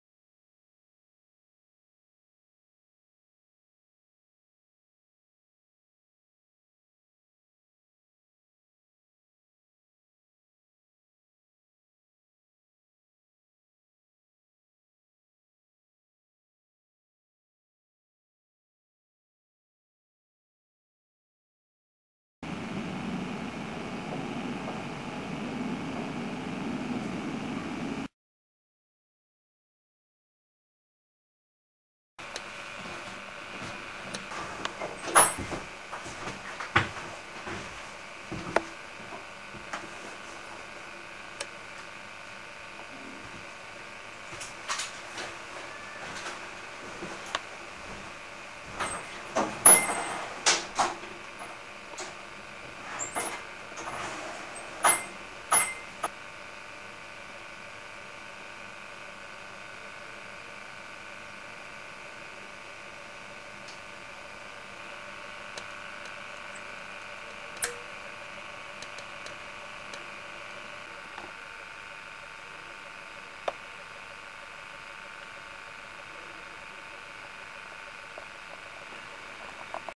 Baby salamander sequel 16 october 2006
First day on land! We have now put him in a square glass world, with stones in the middle. Once we put him in the new water, he swam to the surface and climbed on the brown rock.